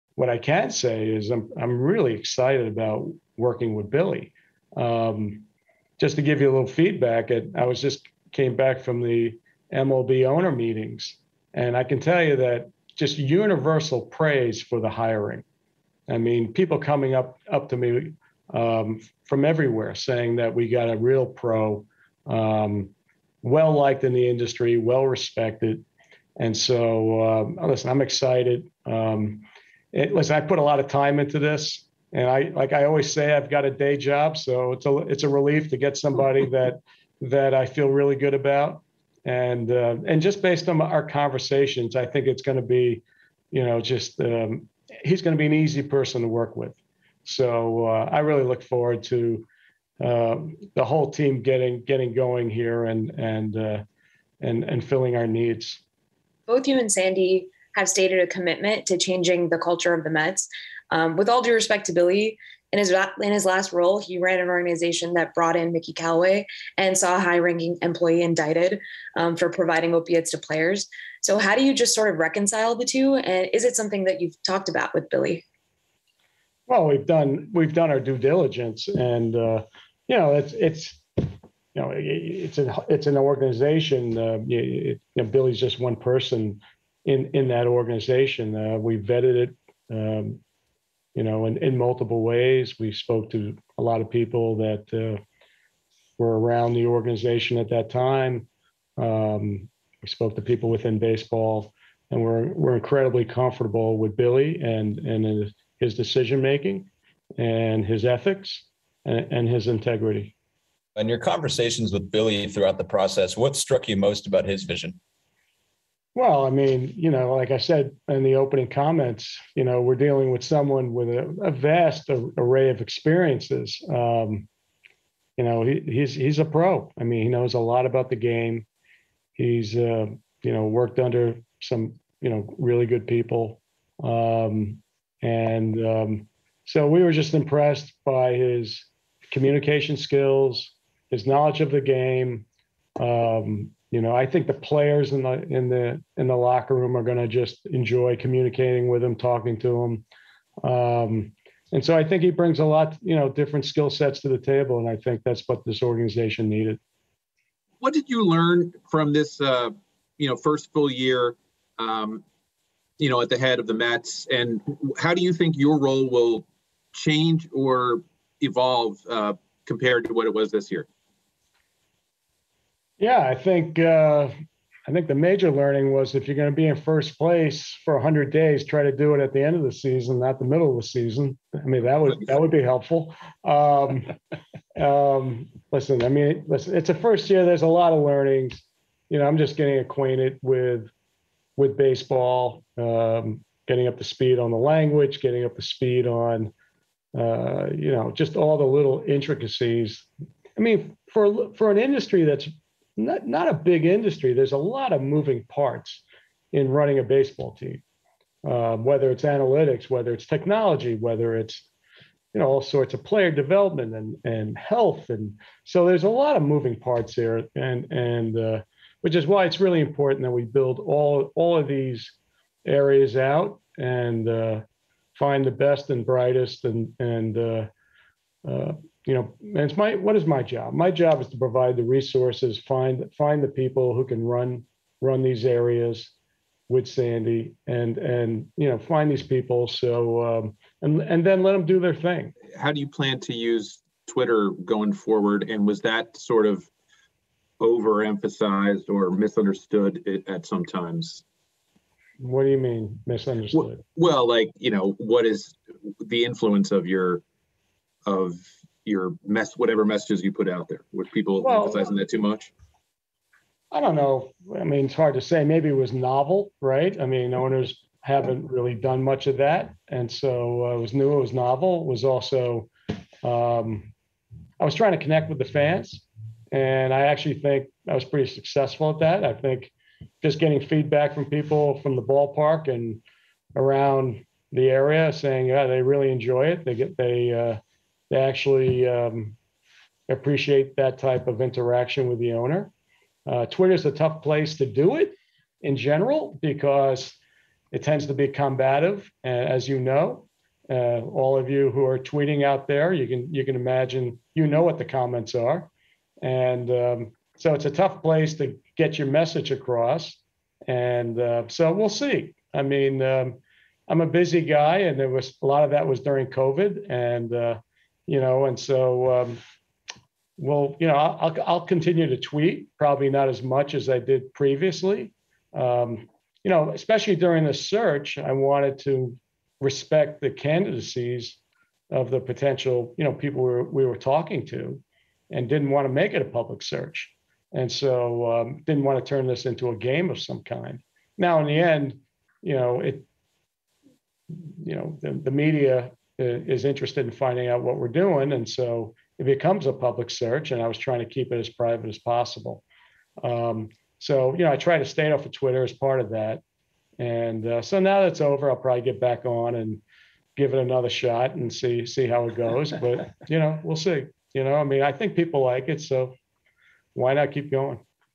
Mets owner Steve Cohen talks new GM Billy Eppler hiring, first year lessons, Twitter plans | SNY
During the news conference introducing Billy Eppler as the next GM of the New York Mets, owner Steve Cohen shared his thoughts on finding his new GM, discussed his process for vetting Eppler before the hire, revealed what he learned in his first year owning the team, and talked about his plans to remain active on Twitter in order to connect with Mets fans.